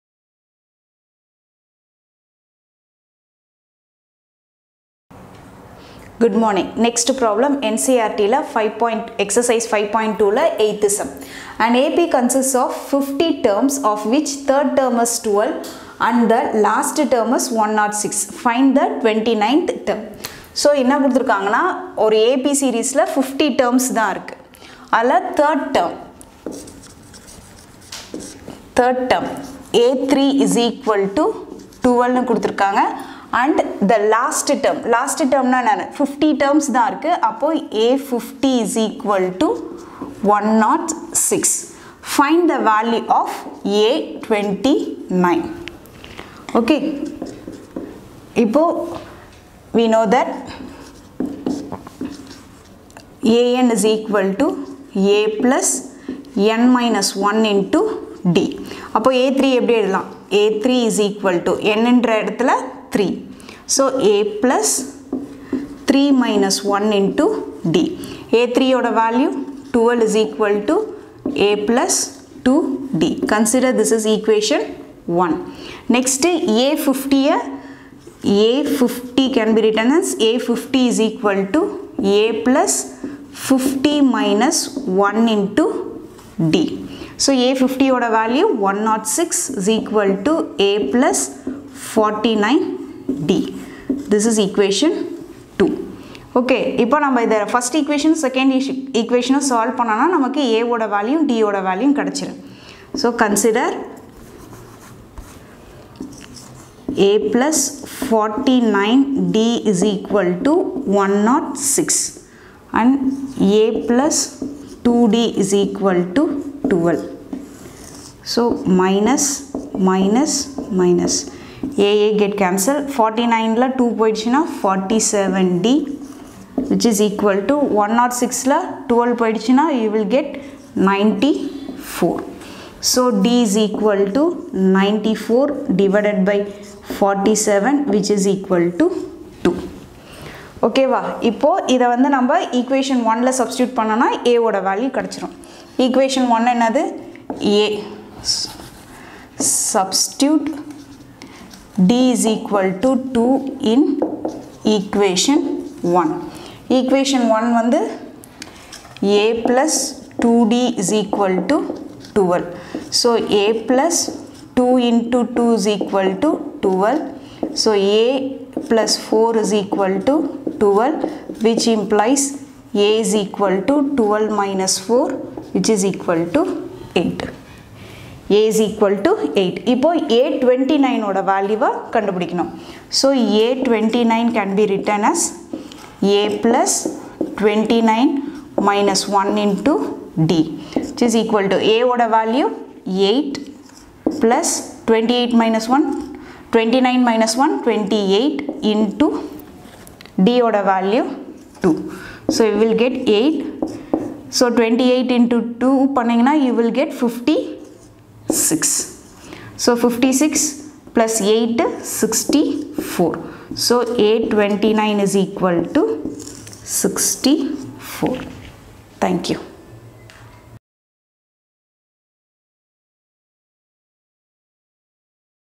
NCERT - Class x - Ex 5.2 - 8 - Page No. 106
# an AP consist of 50 terms of which 3rd term is 12 and the last term is 106. find the 29th term.